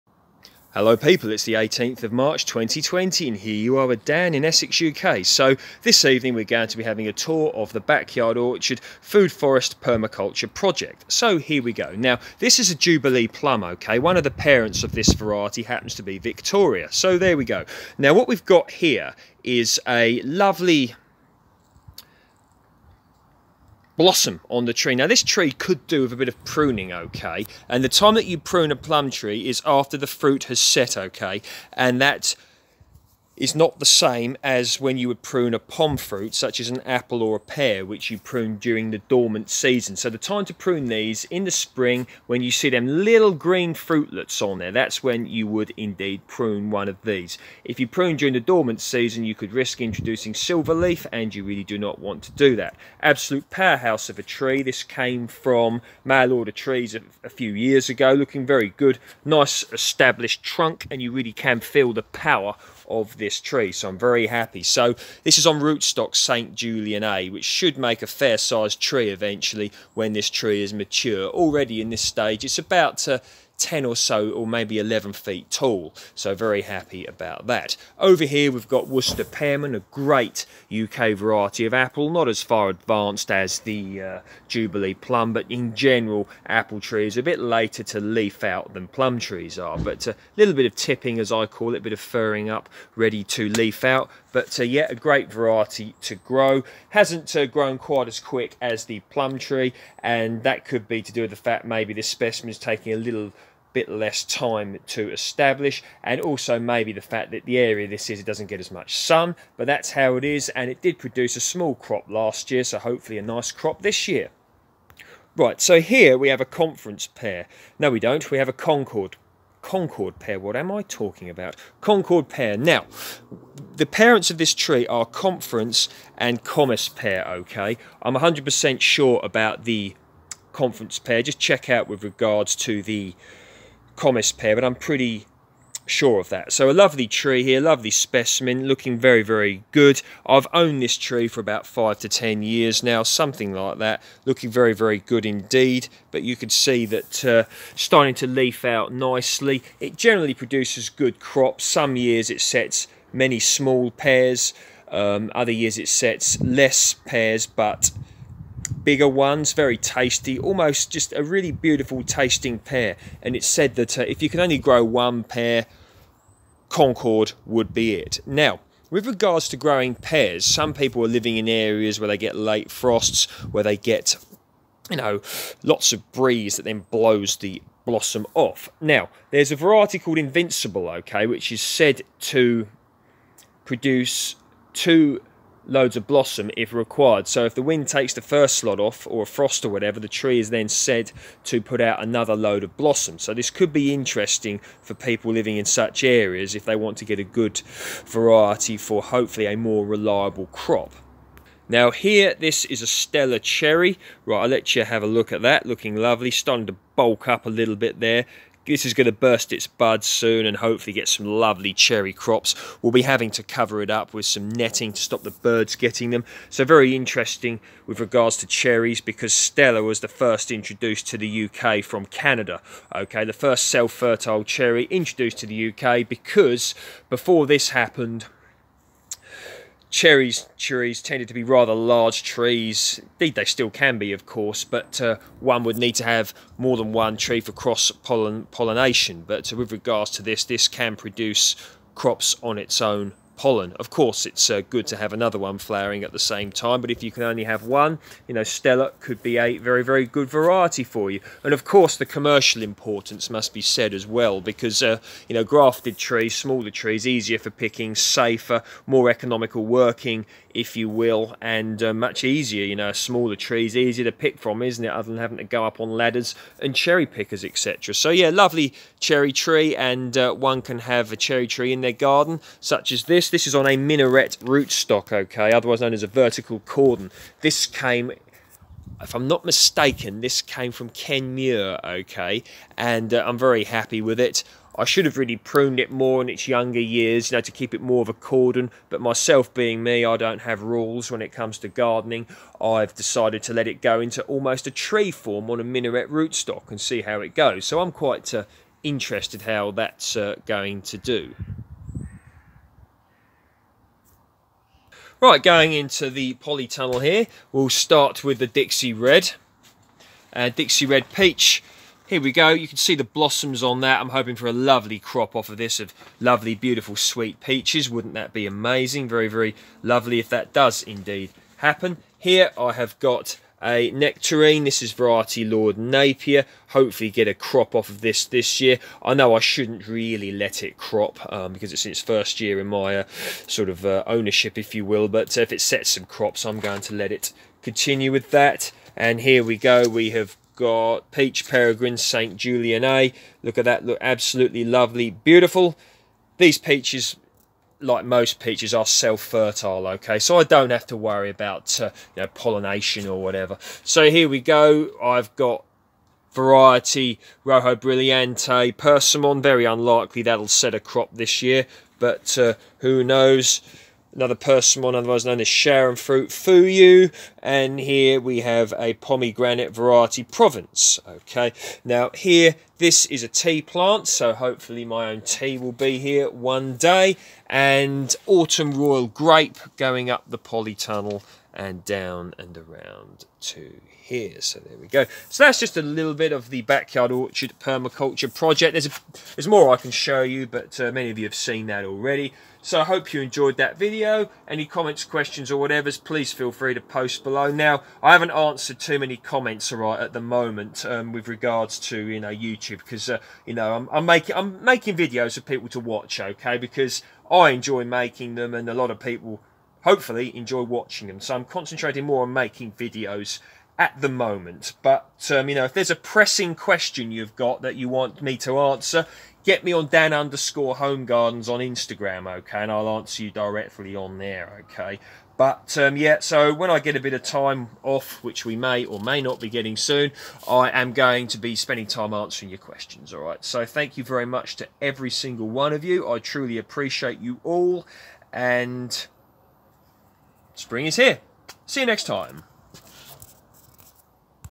Springtime Food forest, backyard orchard, permaculture
A look around a food forest in the South East of the UK.